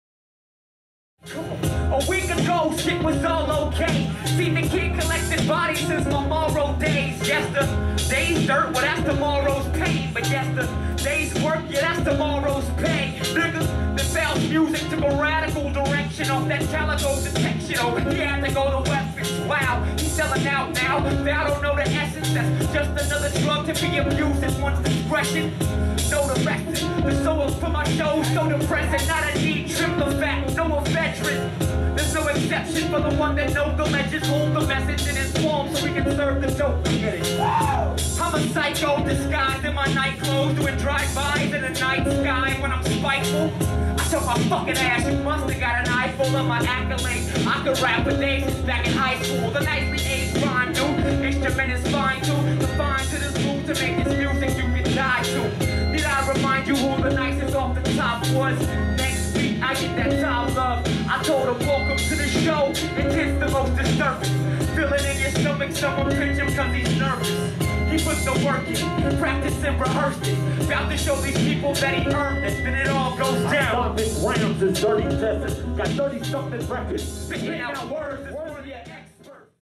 Phal Kilmer live in Tempe, AZ (November 2017)
Phal Kilmer (DFS/Gladiator Pen) live at Tempe Tavern w/ Floyd Write (November 2017).